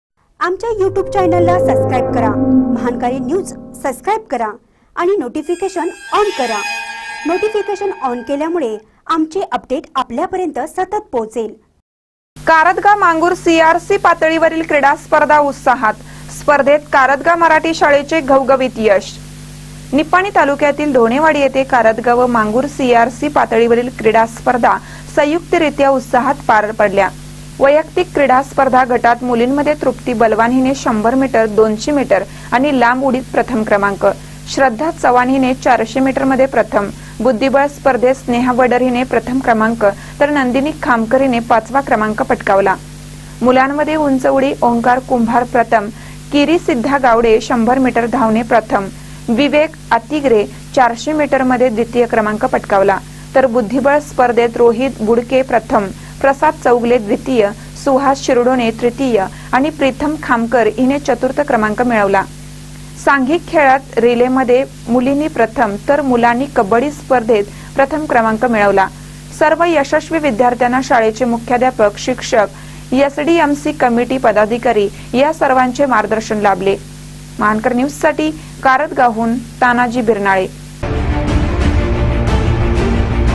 कारदगा मांगूर सीआरसी पातळीवरील क्रिडा स्पर्धा उत्साहात
कारदगा मांगूर सीआरसी पातळीवरील क्रिडा स्पर्धा उत्साहात : स्पर्धे कारदगा मराठी शाळेचे घवघवीत यश.